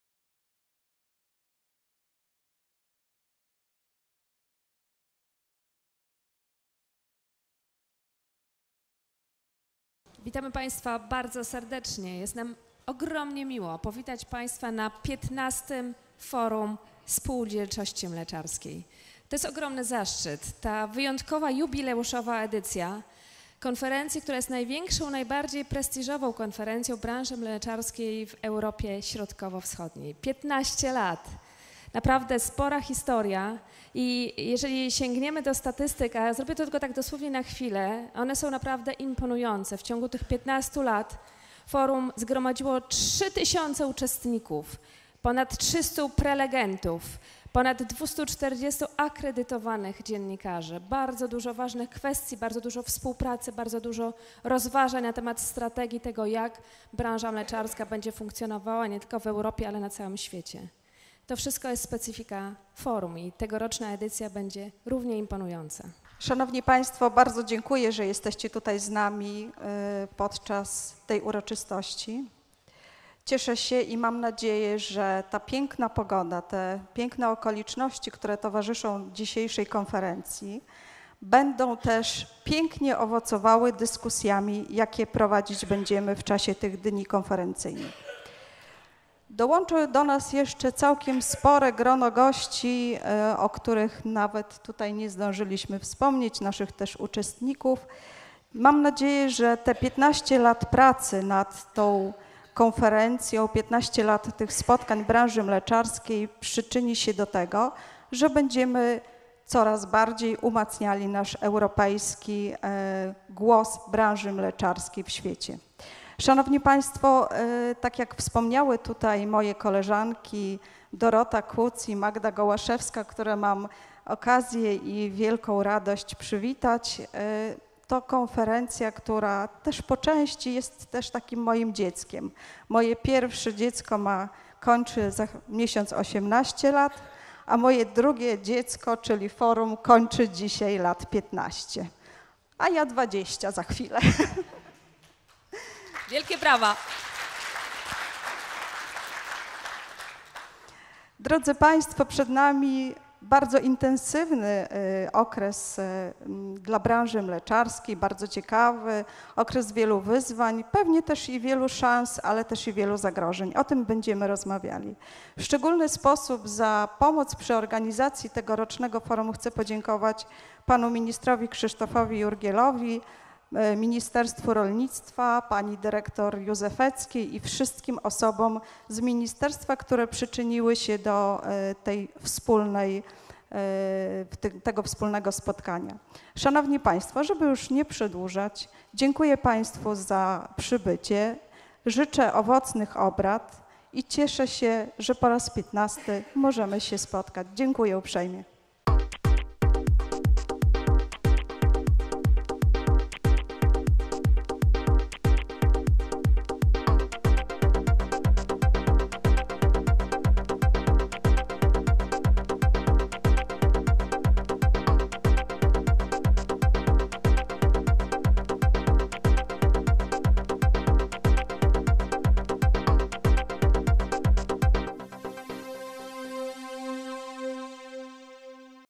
Otwarcie XV Forum Spółdzielczości Mleczarskiej
W dniach 18-20 października 2017 w Hotelu Narvil w Serocku odbyło się XV Forum Spółdzielczości Mleczarskiej. Gośćmi honorowymi Forum byli: Phil Hogan – unijny komisarz ds. rolnictwa i rozwoju obszarów wiejskich i minister rolnictwa I rozwoju wsi – Krzysztof Jurgiel. Forum prowadziły i powitały gości Dorota Kuc i Magda Gołaszewska dziennikarki TVP Białystok. Oficjalnego otwarcia dokonała Agnieszka Maliszewska – dyrektor biura Polskiej Izby Mleka.